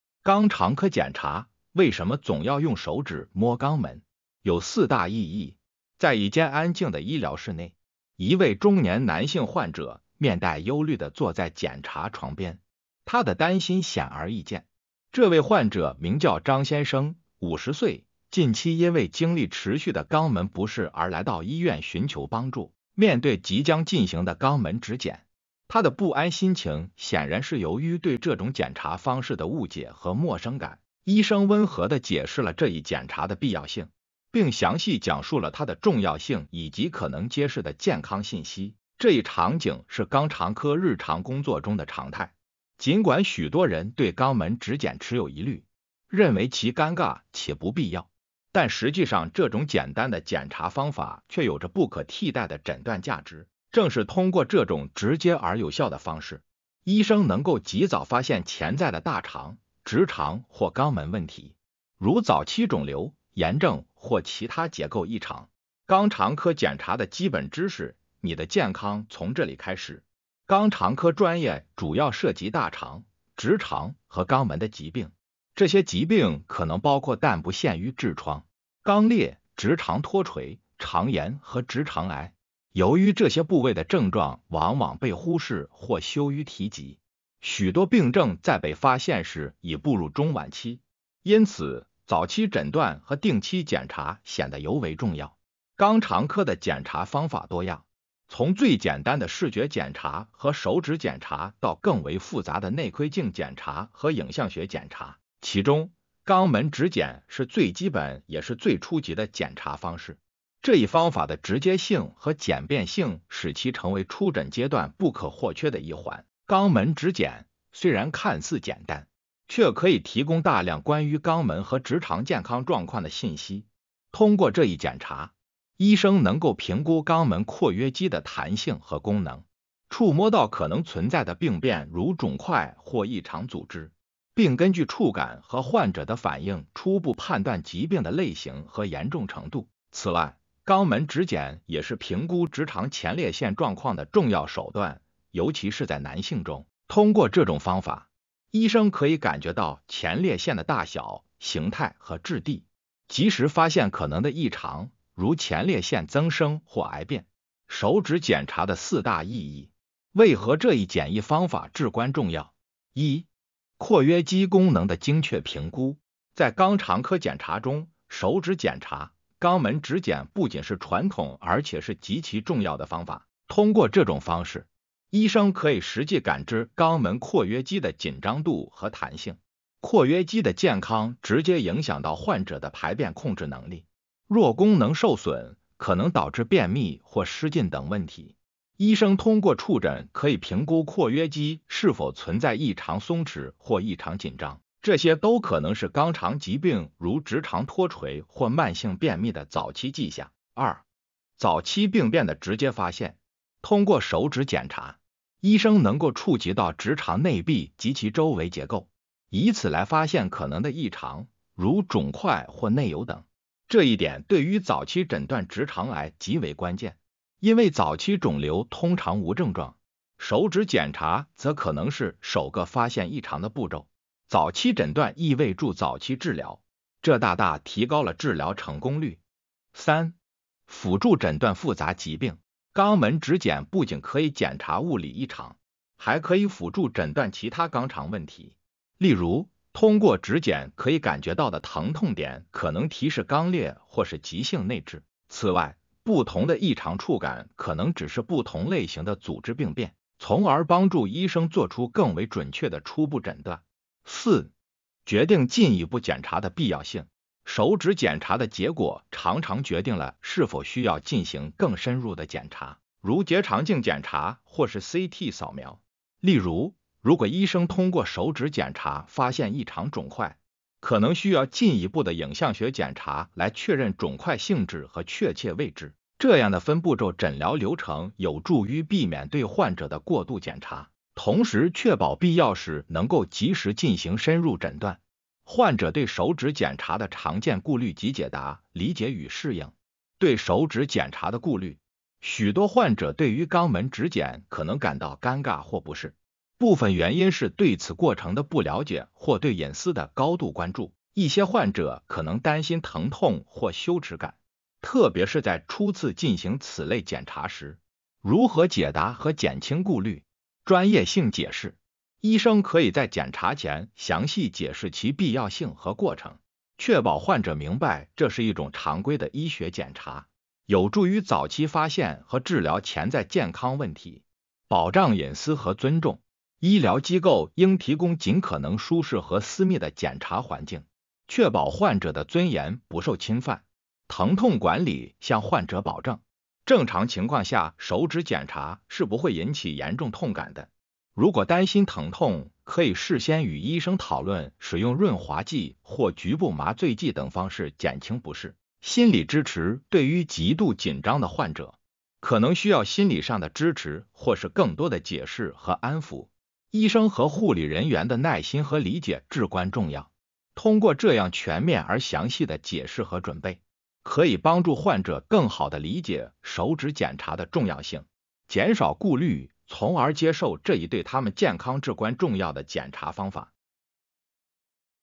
肛腸科檢查，為什麽總要用手指摸肛門?有4大意義
在一間安靜的醫療室內，一位中年男性患者面帶憂慮地坐在檢查床邊，他的擔心顯而易見。這位患者名叫張先生，50歲，近期因為經歷持續的肛門不適而來到醫院尋求幫助。面對即將進行的肛門指檢，他的不安心情顯然是由於對這種檢查方式的誤解和陌生感。醫生溫和地解釋了這一檢查的必要性，並詳細講述了它的重要性以及可能揭示的健康信息。

這一場景是肛腸科日常工作中的常態。盡管許多人對肛門指檢持有疑慮，認為其尷尬且不必要，但實際上這種簡單的檢查方法卻有著不可替代的診斷價值。正是通過這種直接而有效的方式，醫生能夠及早發現潛在的大腸、直腸或肛門問題，如早期腫瘤、炎癥或其他結構異常。

肛腸科檢查的基本知識：你的健康從這里開始
肛腸科專業主要涉及大腸、直腸和肛門的疾病。這些疾病可能包括但不限於痔瘡、肛裂、直腸脫垂、腸炎和直腸癌。由於這些部位的癥狀往往被忽視或羞於提及，許多病癥在被發現時已步入中晚期。因此，早期診斷和定期檢查顯得尤為重要。

肛腸科的檢查方法多樣，從最簡單的視覺檢查和手指檢查到更為覆雜的內窺鏡檢查和影像學檢查。其中，肛門指檢是最基本也是最初級的檢查方式。這一方法的直接性和簡便性使其成為初診階段不可或缺的一環。

肛門指檢，雖然看似簡單，卻可以提供大量關於肛門和直腸健康狀況的信息。通過這一檢查，醫生能夠評估肛門括約肌的彈性和功能，觸摸到可能存在的病變如腫塊或異常組織，並根據觸感和患者的反應初步判斷疾病的類型和嚴重程度。

此外，肛門指檢也是評估直腸前列腺狀況的重要手段，尤其是在男性中。通過這種方法，醫生可以感覺到前列腺的大小、形態和質地，及時發現可能的異常，如前列腺增生或癌變。

手指檢查的四大意義：為何這一簡易方法至關重要
1. 括約肌功能的精確評估

在肛腸科檢查中，手指檢查（肛門指檢）不僅是傳統而且是極其重要的方法。通過這種方式，醫生可以實際感知肛門括約肌的緊張度和彈性。括約肌的健康直接影響到患者的排便控制能力，若功能受損，可能導致便秘或失禁等問題。醫生通過觸診可以評估括約肌是否存在異常松弛或異常緊張，這些都可能是肛腸疾病如直腸脫垂或慢性便秘的早期跡象。

2. 早期病變的直接發現

通過手指檢查，醫生能夠觸及到直腸內壁及其周圍結構，以此來發現可能的異常，如腫塊或內疣等。這一點對於早期診斷直腸癌極為關鍵，因為早期腫瘤通常無癥狀，手指檢查則可能是首個發現異常的步驟。早期診斷意味著早期治療，這大大提高了治療成功率。

3. 輔助診斷覆雜疾病

肛門指檢不僅可以檢查物理異常，還可以輔助診斷其他肛腸問題。例如，通過指檢可以感覺到的疼痛點可能提示肛裂或是急性內痔。此外，不同的異常觸感可能指示不同類型的組織病變，從而幫助醫生做出更為準確的初步診斷。

4. 決定進一步檢查的必要性

手指檢查的結果常常決定了是否需要進行更深入的檢查，如結腸鏡檢查或是CT掃描。例如，如果醫生通過手指檢查發現異常腫塊，可能需要進一步的影像學檢查來確認腫塊性質和確切位置。這樣的分步驟診療流程有助於避免對患者的過度檢查，同時確保必要時能夠及時進行深入診斷。

患者對手指檢查的常見顧慮及解答：理解與適應
對手指檢查的顧慮

許多患者對於肛門指檢可能感到尷尬或不適，部分原因是對此過程的不了解或對隱私的高度關注。一些患者可能擔心疼痛或羞恥感，特別是在初次進行此類檢查時。

如何解答和減輕顧慮

專業性解釋：醫生可以在檢查前詳細解釋其必要性和過程，確保患者明白這是一種常規的醫學檢查，有助於早期發現和治療潛在健康問題。

保障隱私和尊重：醫療機構應提供盡可能舒適和私密的檢查環境，確保患者的尊嚴不受侵犯。

疼痛管理：向患者保證，正常情況下手指檢查是不會引起嚴重痛感的。如果擔心疼痛，可以事先與醫生討論使用潤滑劑或局部麻醉劑等方式減輕不適。

心理支持：對於極度緊張的患者，可能需要心理上的支持或是更多的解釋和安撫。醫生和護理人員的耐心和理解至關重要。

通過這樣全面而詳細的解釋和準備，可以幫助患者更好地理解手指檢查的重要性，減少顧慮，從而接受這一對他們健康至關重要的檢查方法。